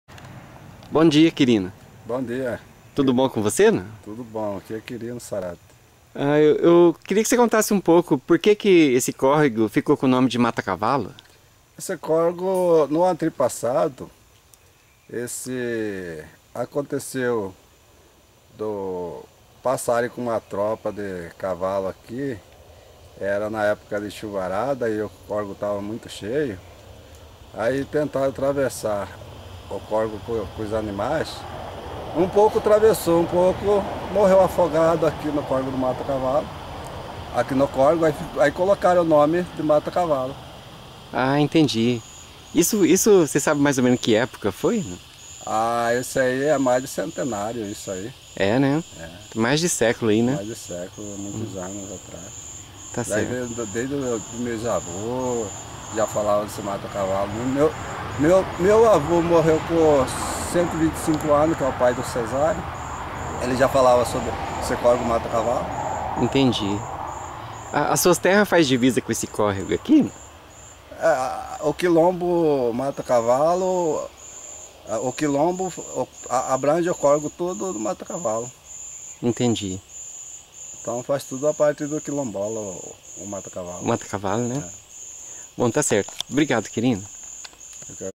CULTURA QUILOMBOLA - Origem do nome da comunidade do Quilombo Mata Cavalo - Prof. Silvânio Barcelos
O Sr. Quirino do Quilombo Mata Cavalo explica o origem do nome de sua comunidade quilombola. Acervo histórico. Prof. Silvânio Barcelos